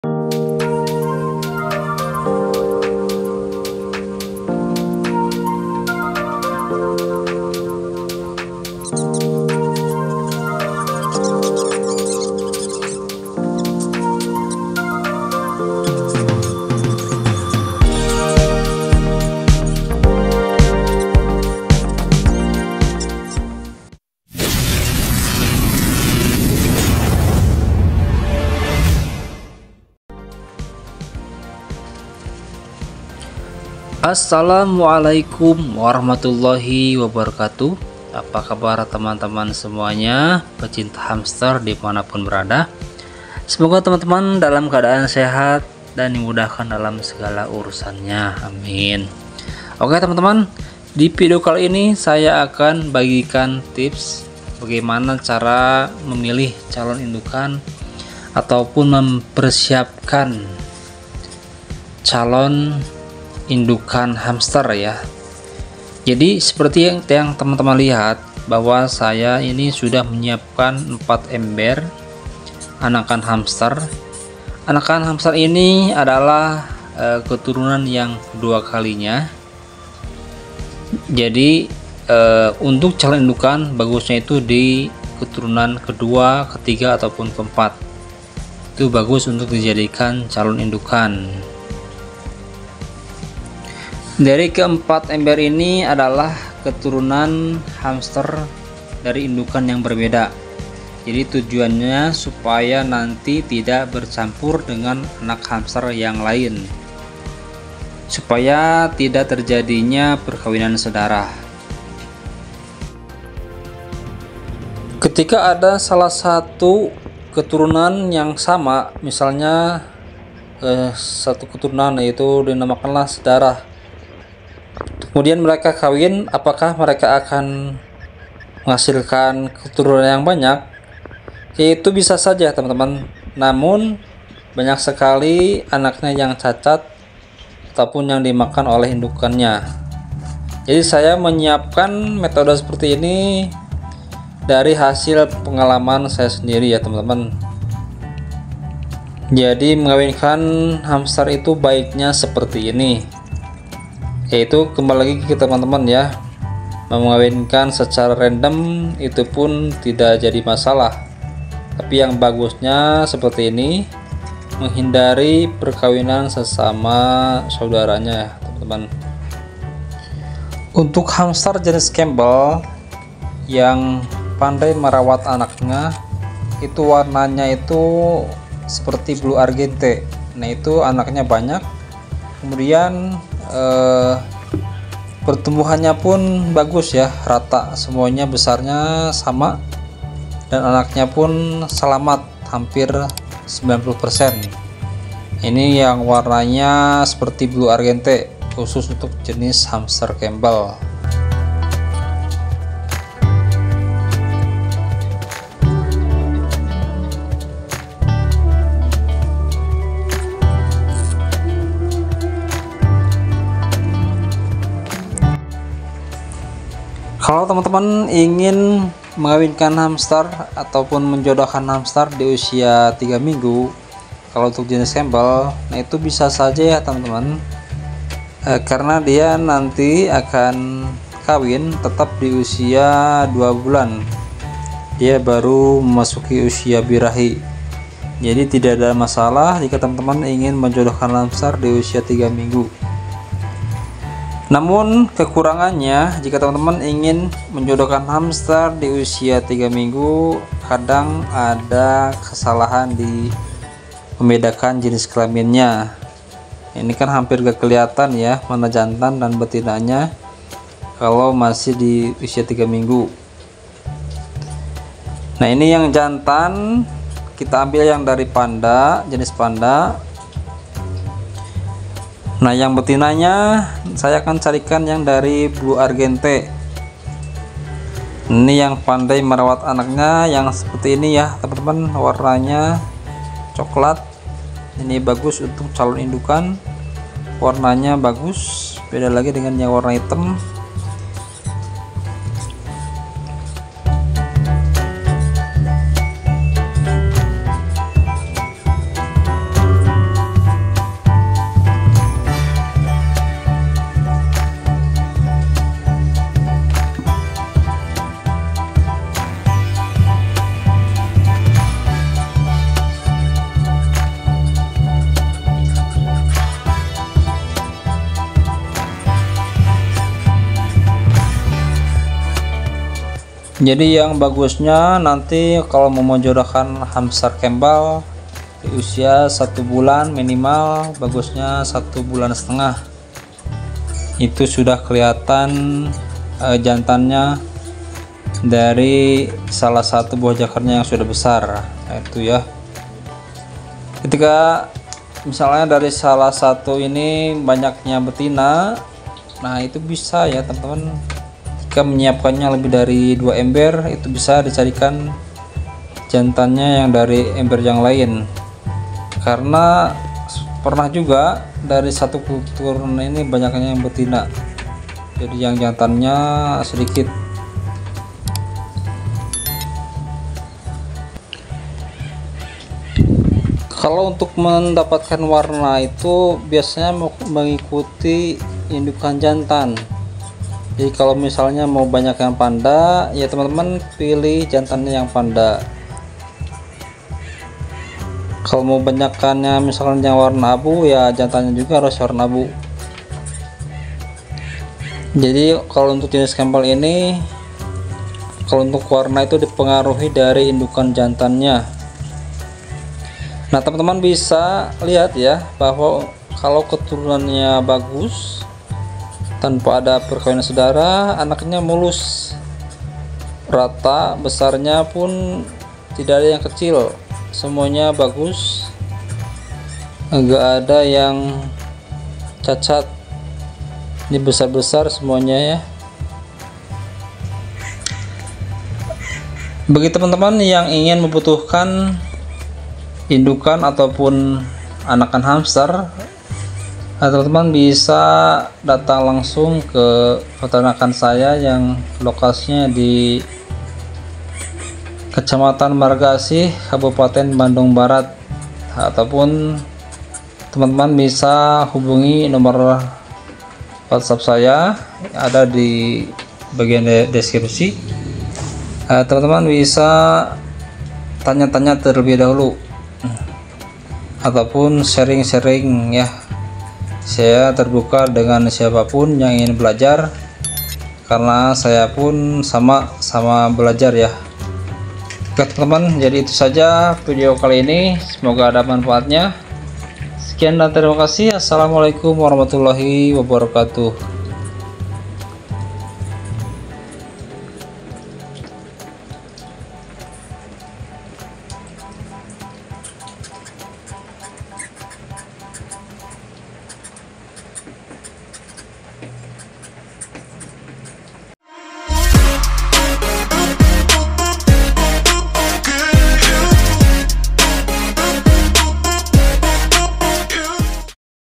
CARA MEMILIH HAMSTER UNTUK CALON INDUKAN YANG BAIK | hamster campbell
CARA MEMILIH HAMSTERS UNTUK CALON INDUKAN YANG BAIK

Tips Memilih Calon Indukan Hamster yang Baik.Agar menghasilkan keturunan yang baik perlu adanya persiapan dan pemilihan bakal calon dari genetika yang bagus.Salah satunya sudah saya praktekan di video ini, supaya gak gagal paham simak terus videonya dari awal  sampai selesai.